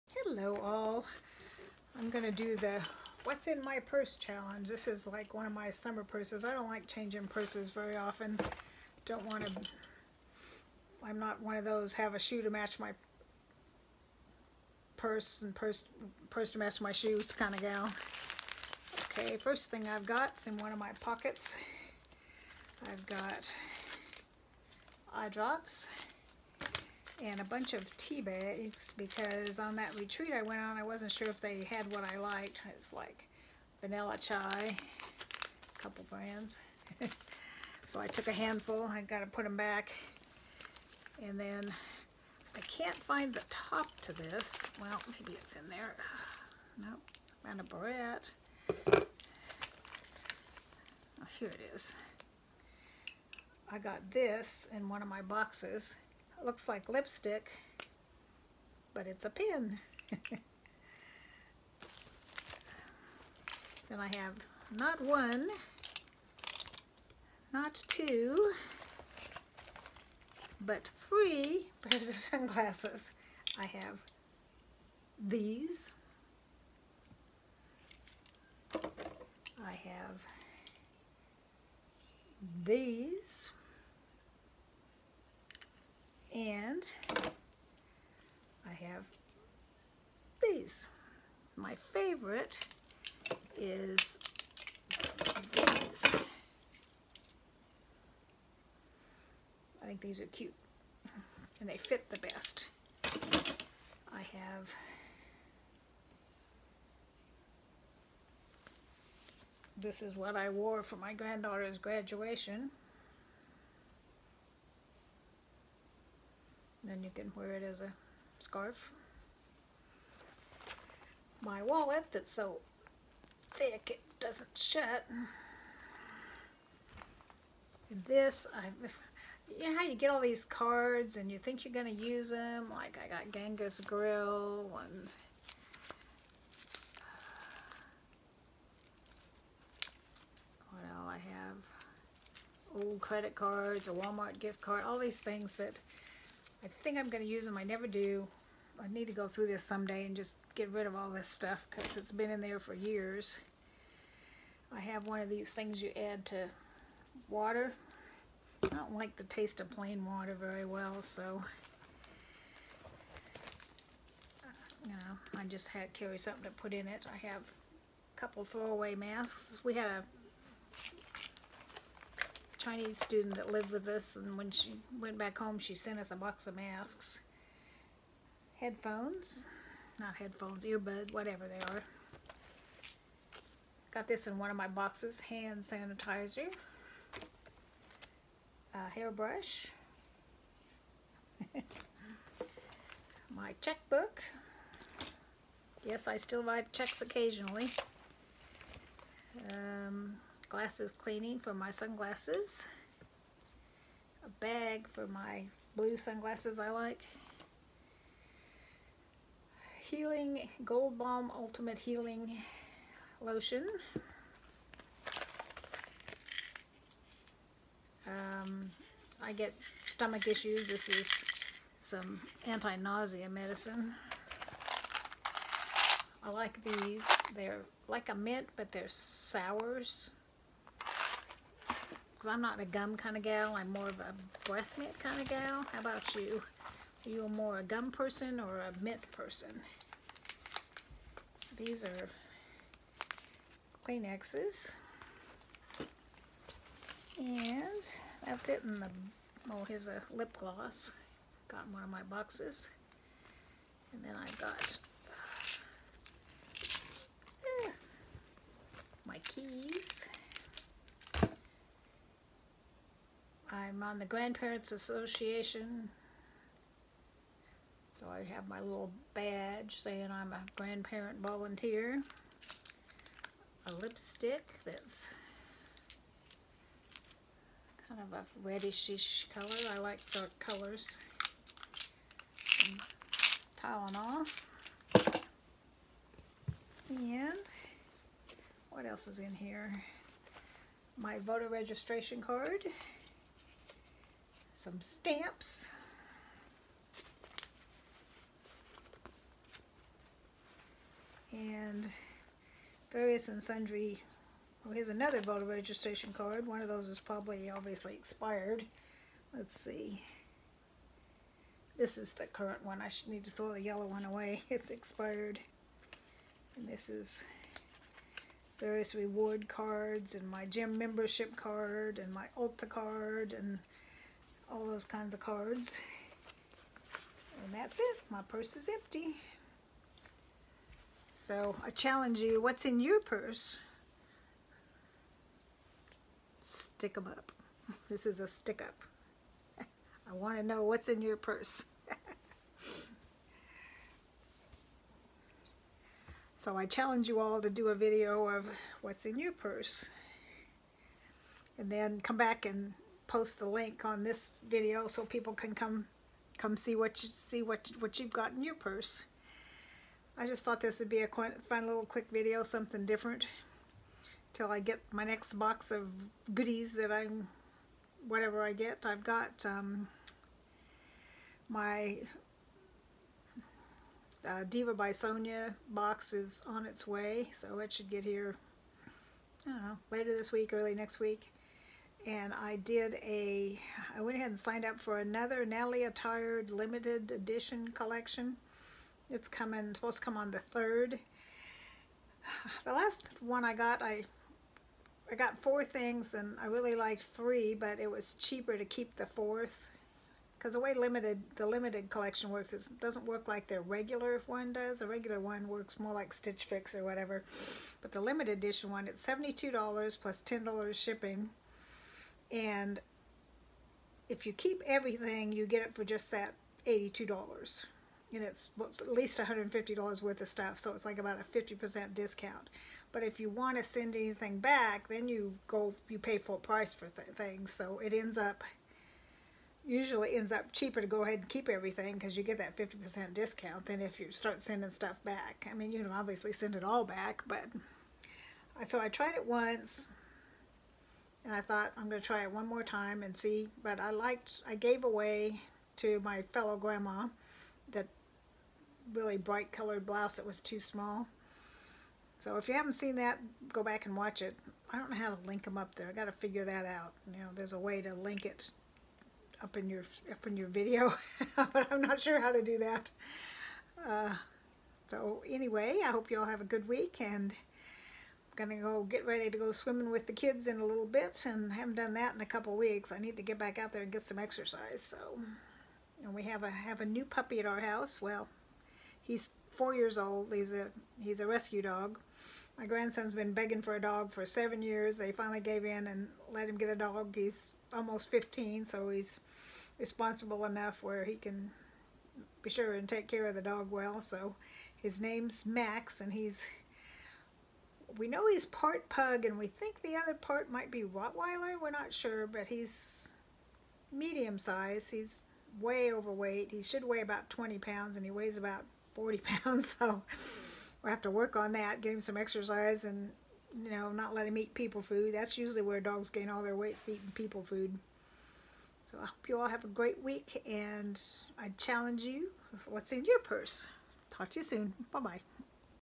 What’s in my purse challenge.
What’s in my purse challenge. Film what’s in your purse then come back and link it here. You would be surprised what a glimpse into your life we get.